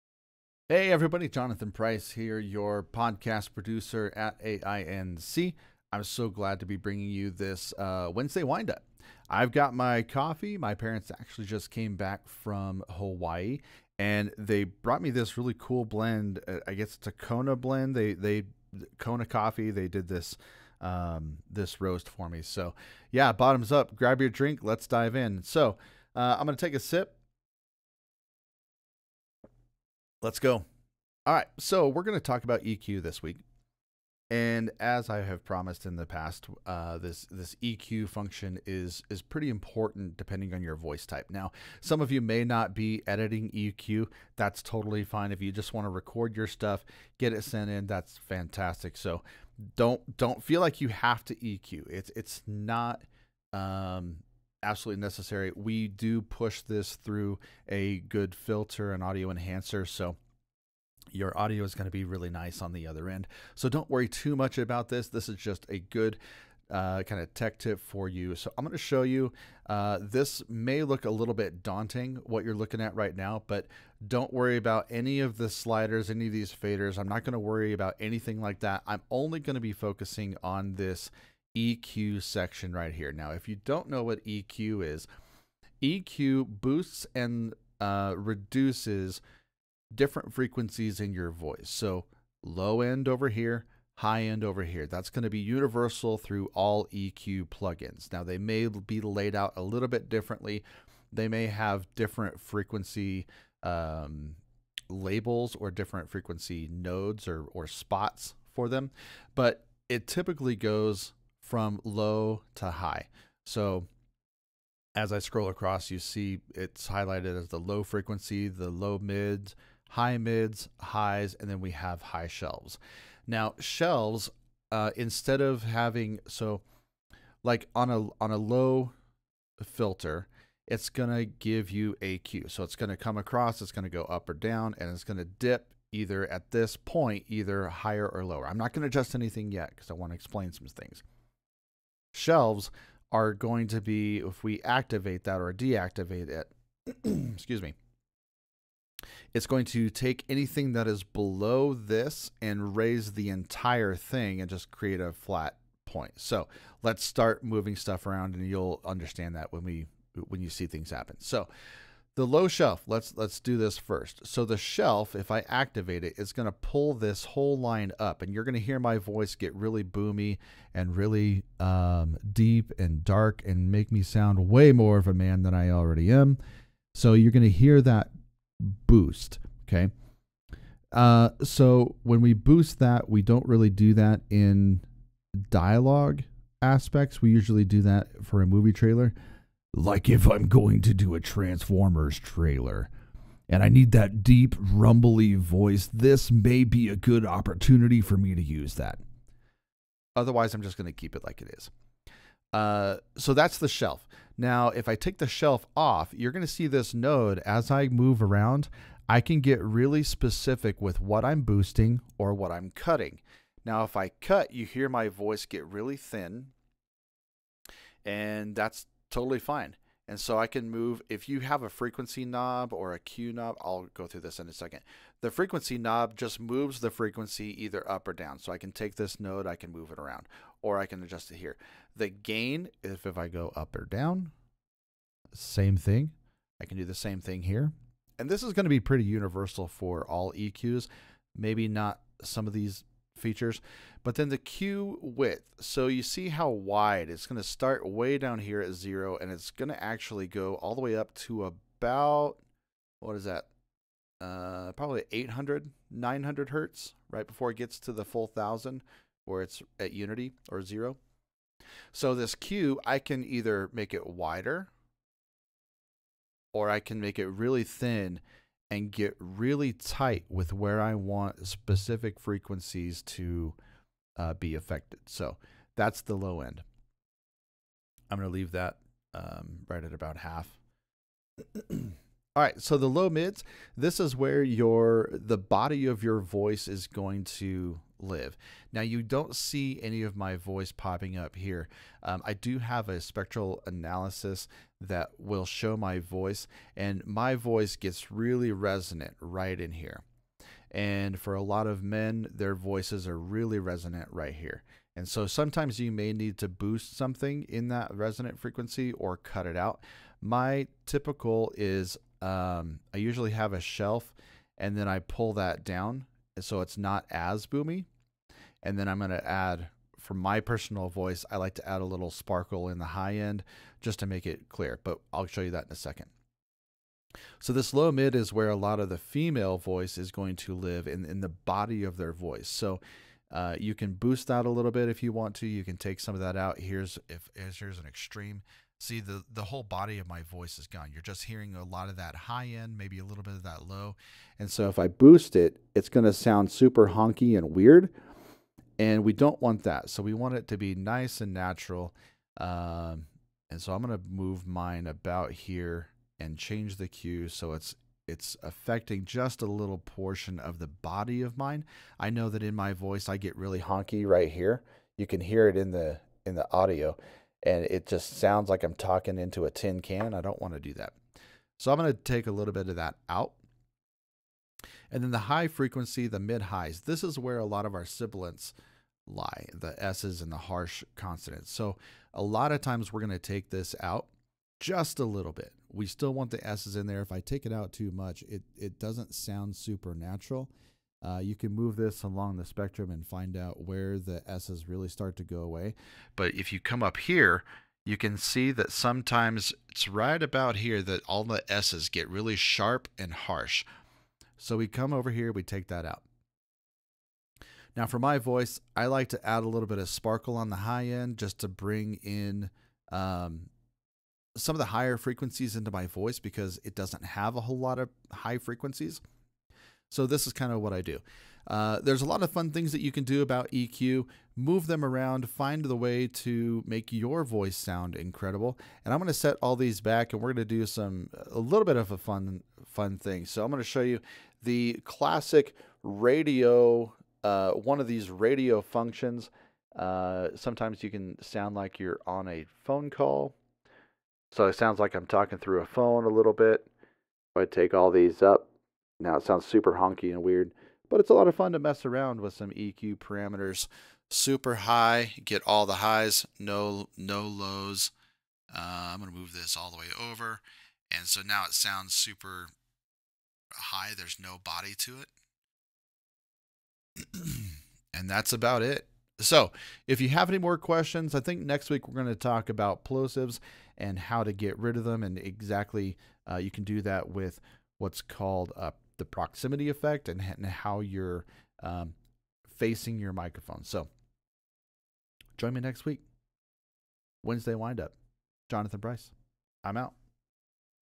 Mastering EQ for Voice & Voiceover | Wednesday Windup with Jonathan Price
Welcome to this week's edition of Wednesday Windup! We're excited to dive deep into the world of EQ with podcast producer, Jonathan Price. Whether you're a budding voice artist or seasoned podcaster, understanding how to tweak frequencies can transform your audio quality and take it to the next level.

🎙️ In this episode, you'll discover:

The fundamentals of EQ and its importance in audio production.
Techniques to boost and reduce frequencies, making your voice sound clear and professional.
Tips and tricks from Jonathan's vast experience in podcast production.
Real-life examples demonstrating the impact of EQ adjustments.

If you're looking to improve your voice or voiceover recordings, this is the episode you won't want to miss.